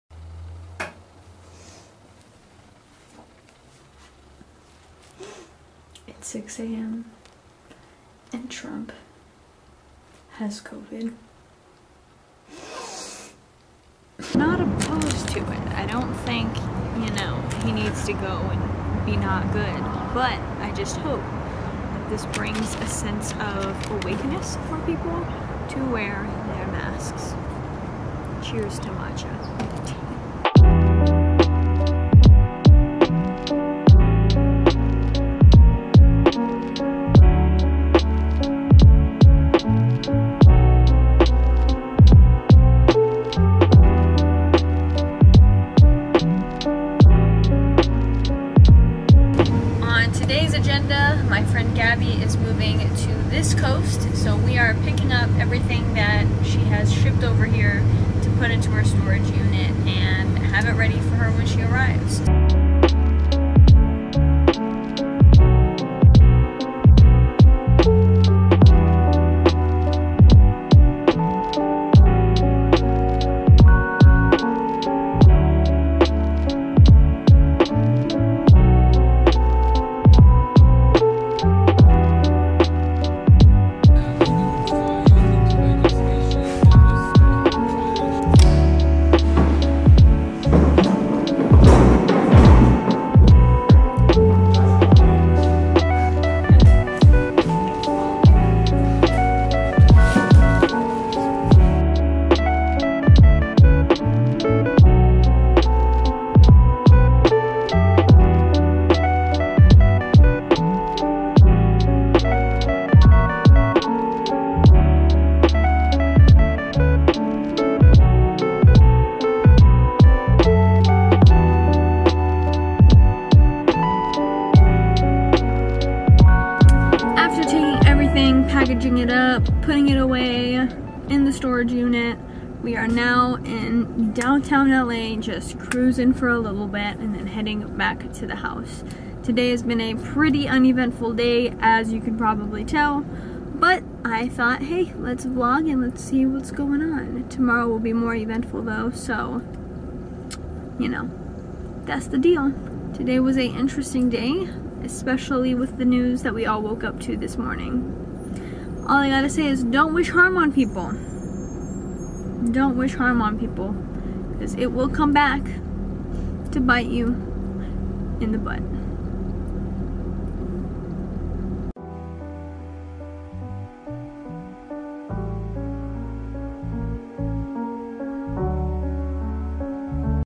Helping a friend move... To my coastline
Not sure the rates, but flying your stuff cross country seems like the best option when moving. after seeing this process wow, what a breeze!